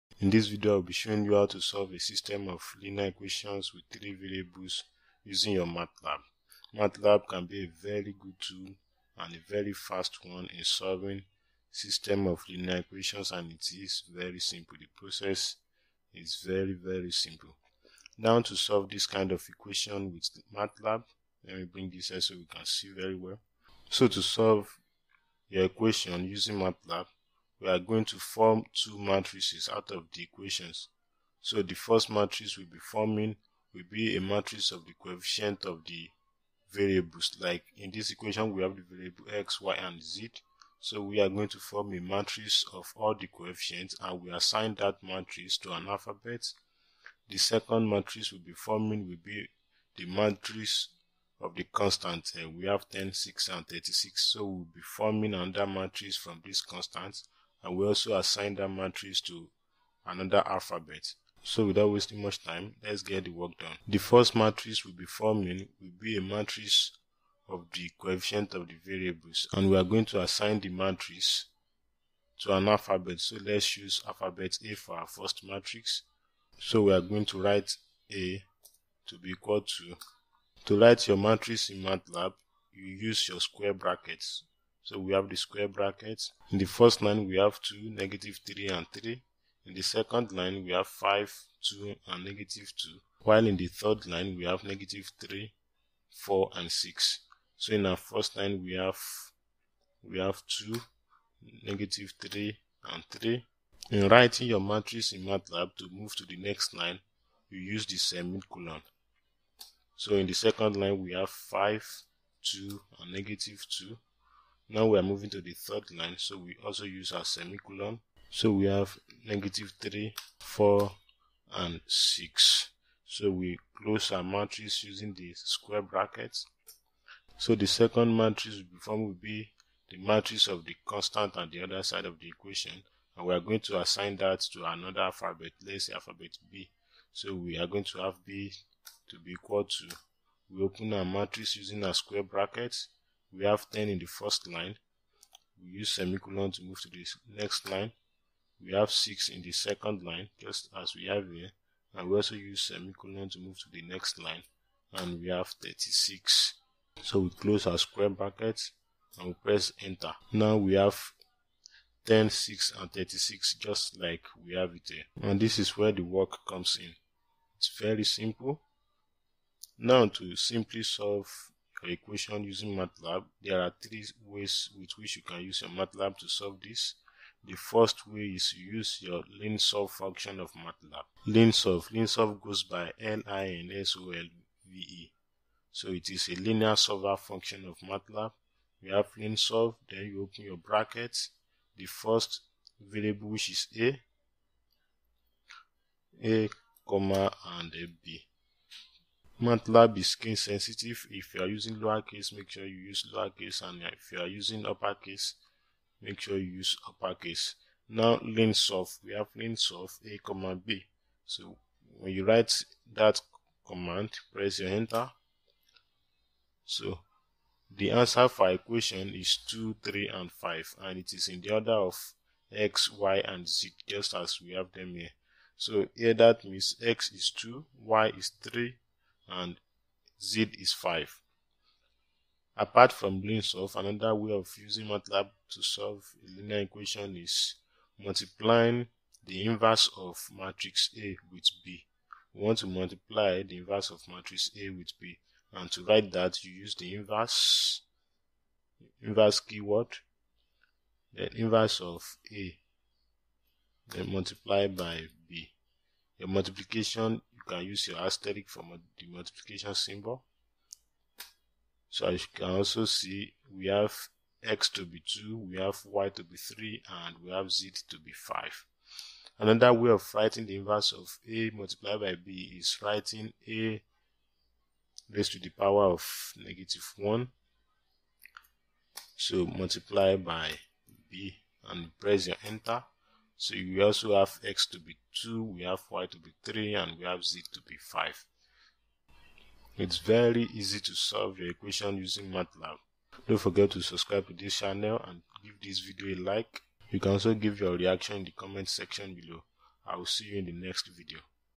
HOW TO USE MATLAB TO SOLVE SYSTEM OF LINEAR EQUATIONS WITH 3 VARIABLES
In this video, you will see how you can solve linear equations with three variables using MATLAB. MATLAB is another nifty software that can help you solve your equations swiftly. Two different methods were used in the video; using the linsolve function of MATLAB and also using the inverse matrix method.

for more topics on math and sciences.
You can also like/follow Easy Math and Circuit on